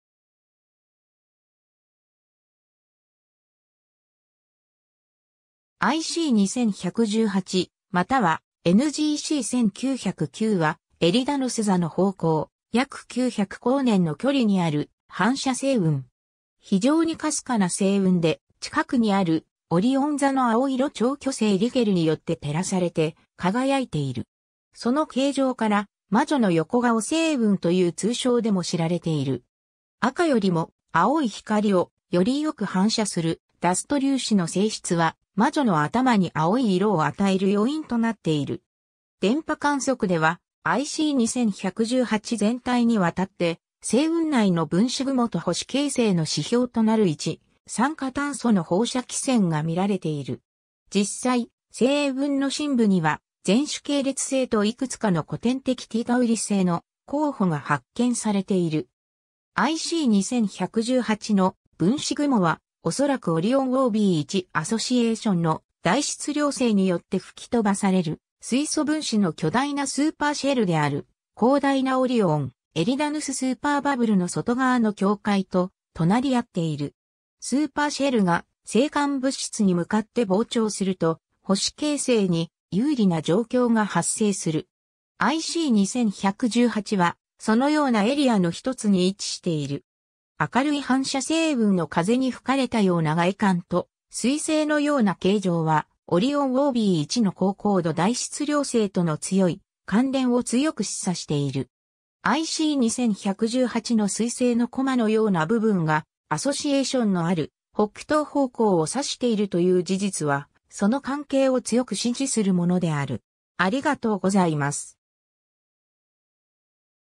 IC 2118
IC 2118またはNGC 1909は、エリダヌス座の方向、約900 光年の距離にある反射星雲。
非常にかすかな星雲で、近くにあるオリオン座の青色超巨星 リゲルによって照らされて輝いている。
その形状から魔女の横顔星雲 (the Witch Head Nebula) という通称でも知られている。
赤よりも青い光をよりよく反射するダスト粒子の性質は、魔女の頭に青い色を与える要因となっている。
電波観測では、IC 2118全体にわたって、星雲内の分子雲と星形成の指標となる一酸化炭素の放射輝線が見られている。
実際、星雲の深部には、前主系列星といくつかの古典的Tタウリ星の候補が発見されている 。
 IC 2118の分子雲は、恐らくオリオンOB1アソシエーションの大質量星によって吹き飛ばされる水素分子の巨大なスーパーシェルである、広大なオリオン-エリダヌススーパーバブルの外側の境界と隣り合っている。
スーパーシェルが星間物質に向かって膨張すると、星形成に有利な状況が発生する。
IC 2118はそのようなエリアの1つに位置している。
明るい反射星雲の風に吹かれたような外観と彗星のような形状は、オリオンOB1の高光度大質量星との強い関連を強く示唆している。
IC2118の彗星のコマのような部分がアソシエーションのある北東方向を指しているという事実は、その関係を強く支持するものである。